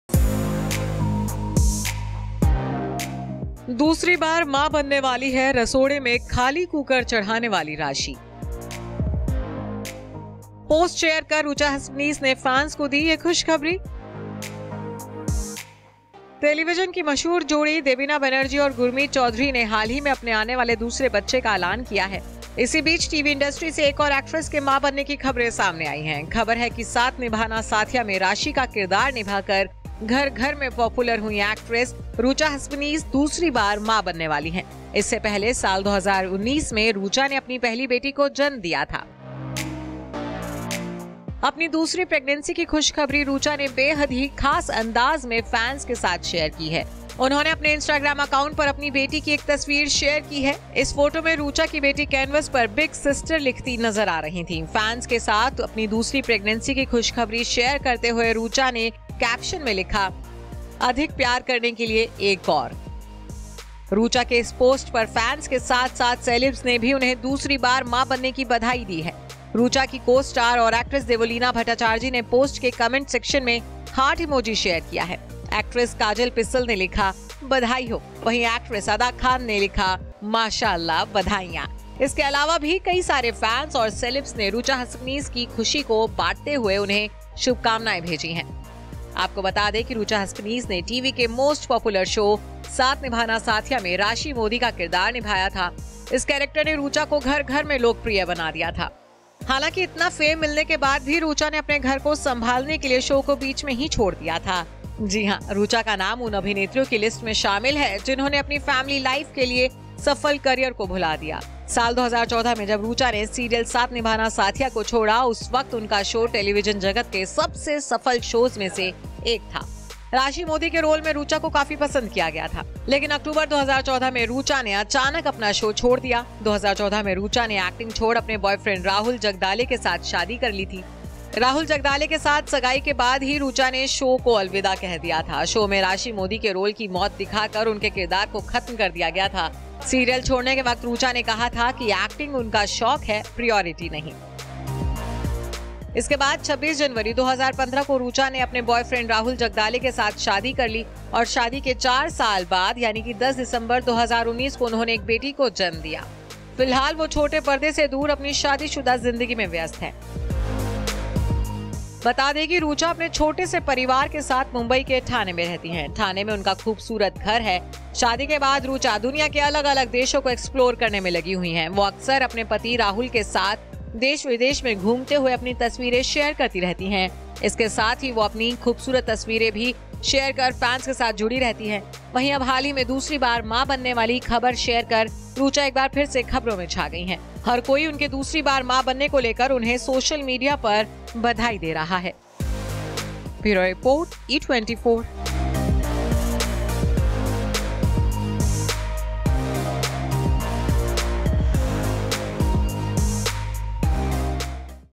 दूसरी बार मां बनने वाली हैं 'साथ निभाना साथिया' की 'राशि', फोटो शेयर कर खुद दी फैंस को गुड न्यूज
टेलीविजन की मशहूर जोड़ी देबिना बनर्जी और गुरमीत चौधरी ने हाल ही में अपने आने वाले दूसरे बच्चे का ऐलान किया हैं। इसी बीच टीवी इंडस्ट्री से एक और एक्ट्रेस की मां बनने की खबर सामने आईं है। खबर है कि ‘साथ निभाना साथिया’ में राशि का किरदीर निभाकर घर-घर में पॅापुलर हुईं एक्ट्रेस रुचा हसबनीस दूसरी बार मां बनने वाली हैं। इससे पहले साल 2019 में रुचा ने अपनी पहली बेटी को जन्म दिया था। अपनी दूसरी प्रेग्नेंसी की खुशखबरी रुचा ने बेहद ही खास अंदाज में फैंस के साथ शेयर की है। उन्होनें अपने इंस्टाग्राम अंकाउट पर अपनी बेटी की एक तस्वीर शेयर की। इस फोटो में रुचा की बेटी कैनवास पर 'बिग सिस्टर' लिखती नजर आ रही थी।